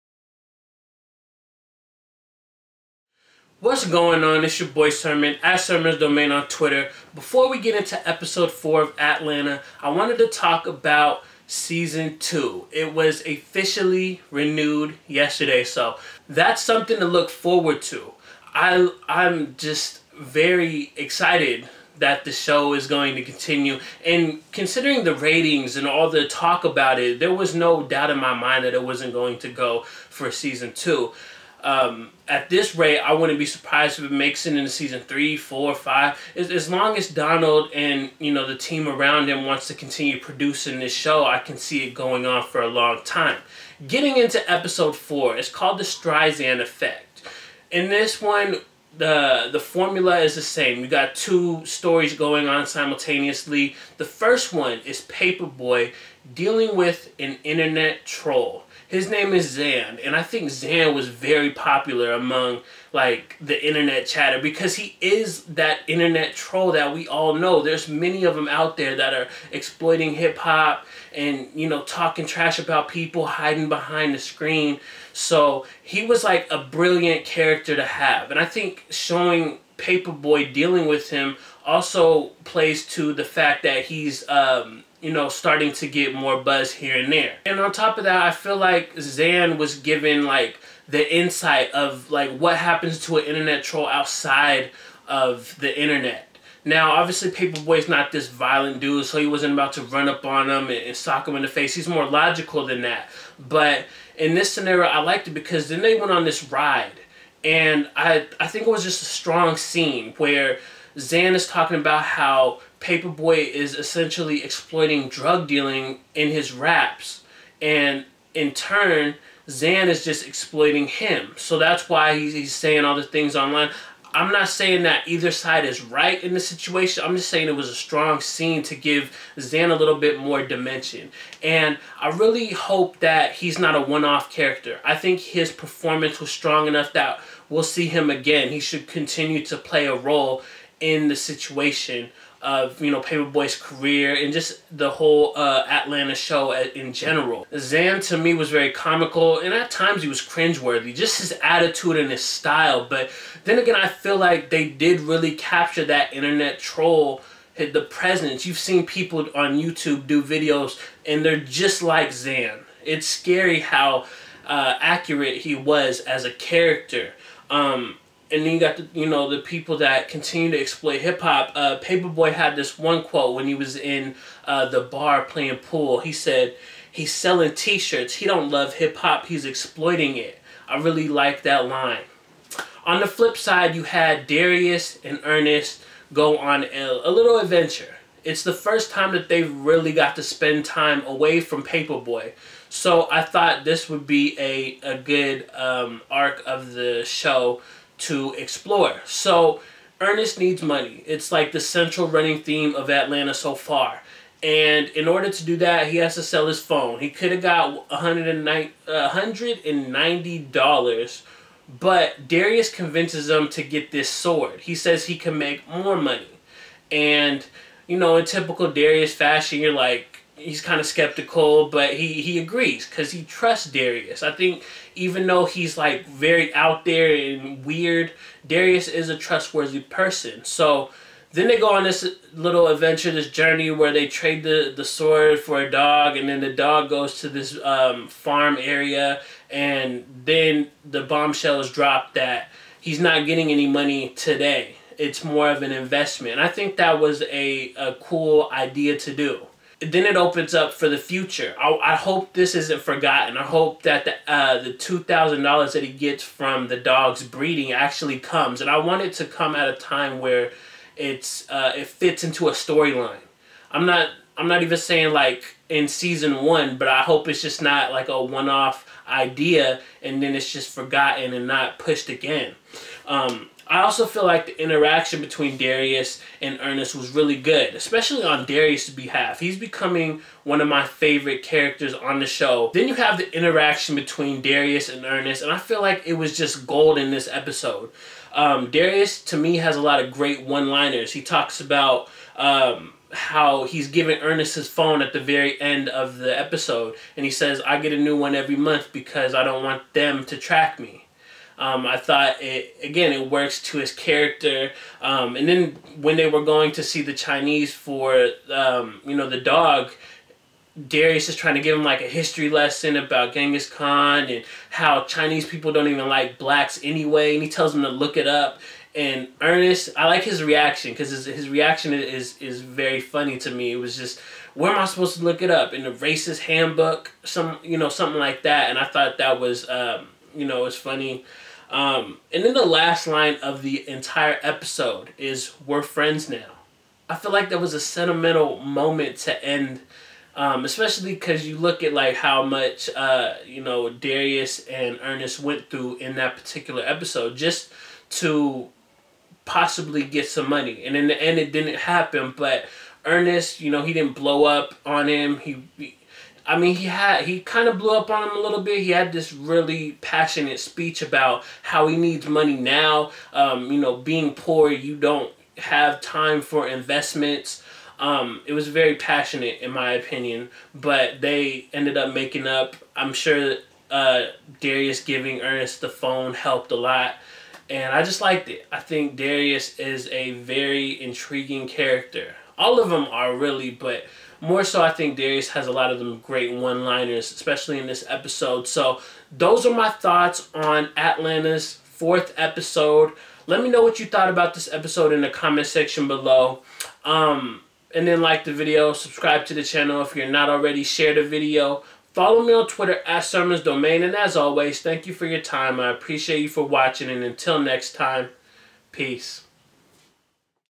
FX's Atlanta - 'The Streisand Effect' (S1, E4 REVIEW)
In episode four, Paper Boi deals with an internet troll and Darius helps Earnest get some (slow) money.